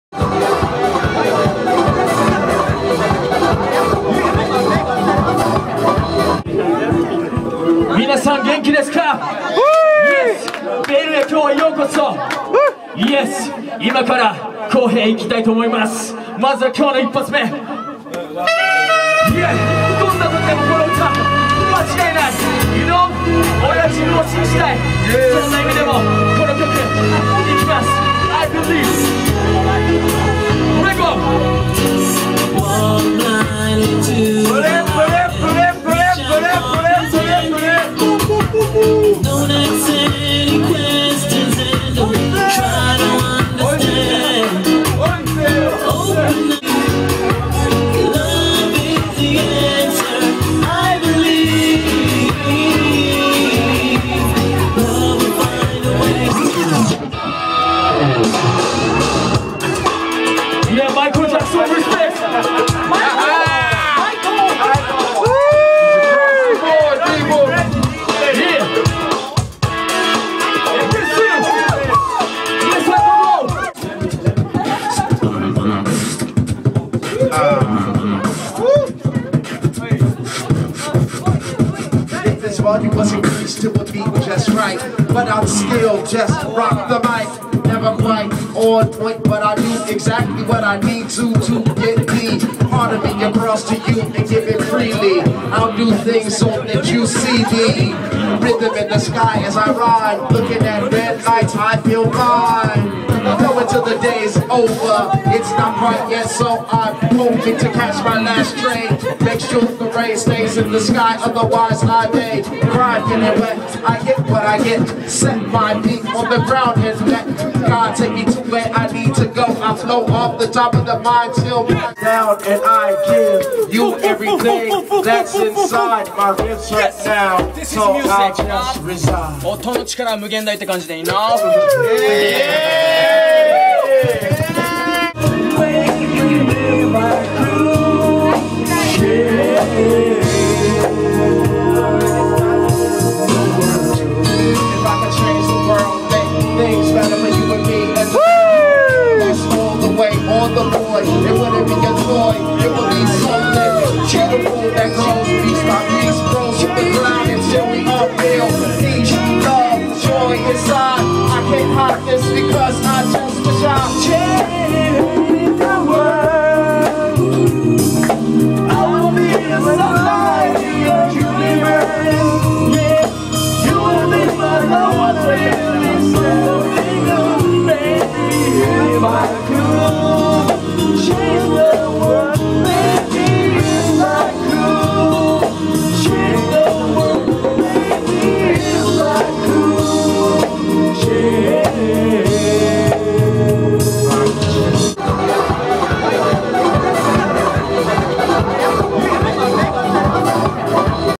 KOHEI AKA THE OUT SPOKEN ONE(Hip Hop Reggae R&B Beat Box FREE STYLE)Tokyo
The possibilities of music are endless . Hip Hop Reggae R&B Beat Box FREE STYLE Tokyo